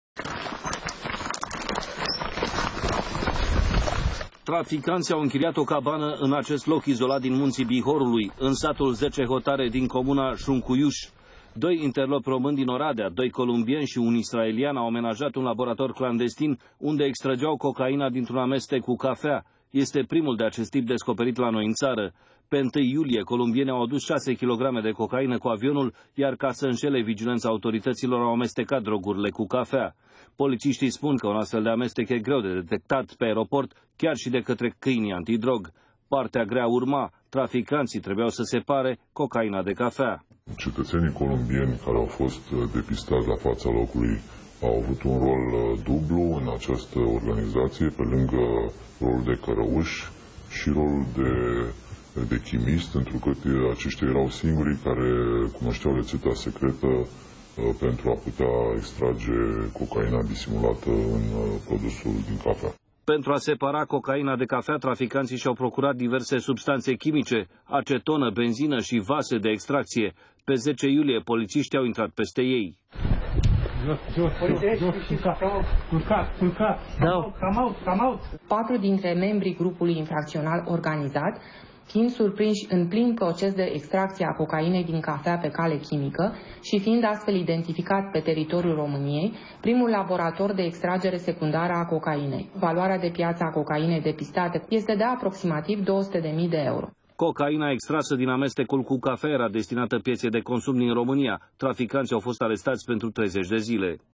Primul laborator de extragere secundară a cocainei din cafea, descoperit în România
Polișiștii antidrog au descoperit primul laborator pentru extracţia cocainei din România. A fost amenajat într-o cabană izolată din Bihor, iar traficantii obţineau cocaina dintr-un amestec de cafea care a fost adus în ţară de doi columbieni. Tot columbienii au adus drogurile în România şi cunoşteau procedura chimică de extracţie. Din reţea mai fac parte doi români şi un israelian.